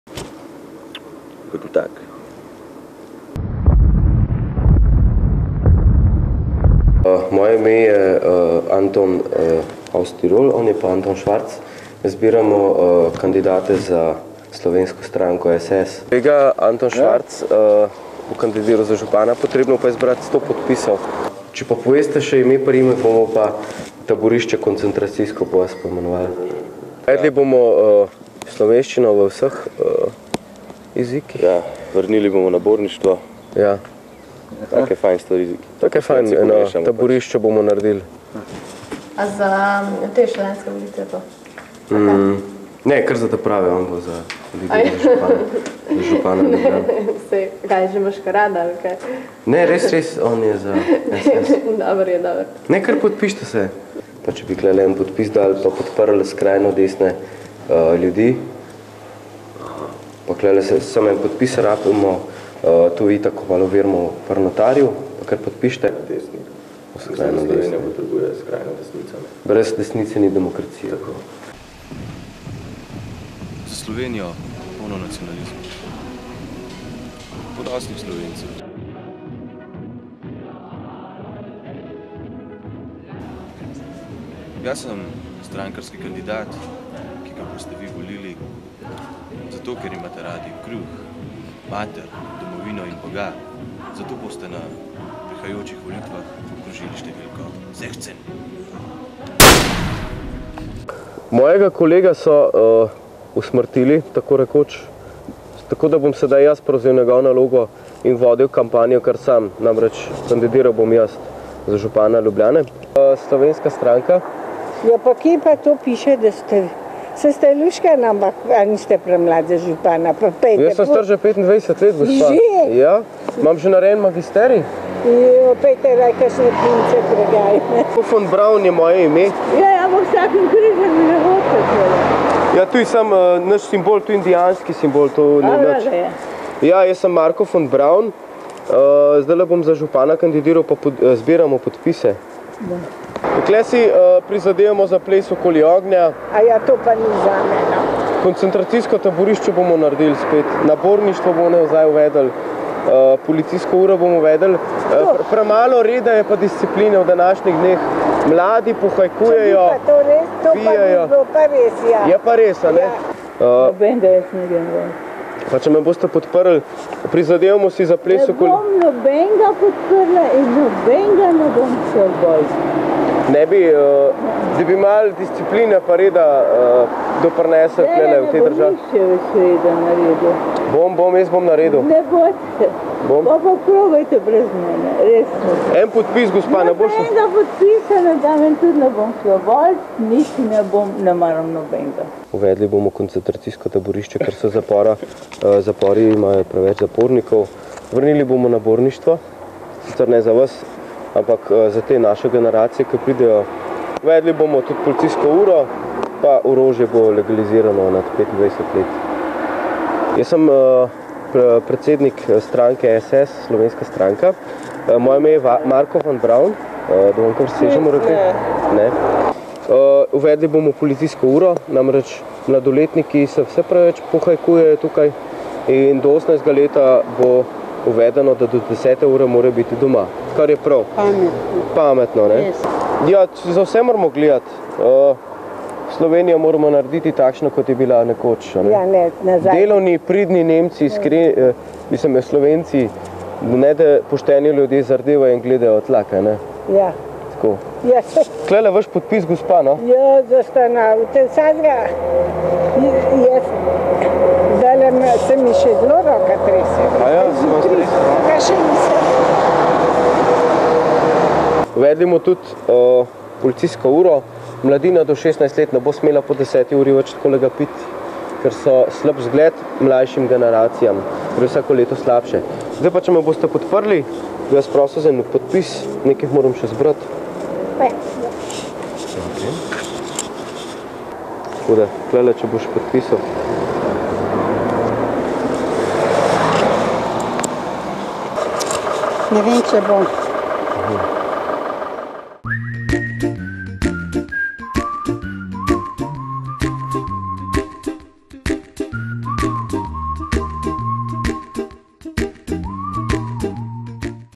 Skrita kamera - ali bi dali glas za...
Koga vse bi si ljudje izbrali ob kriznih časih, da bi nas rešil pred krizo.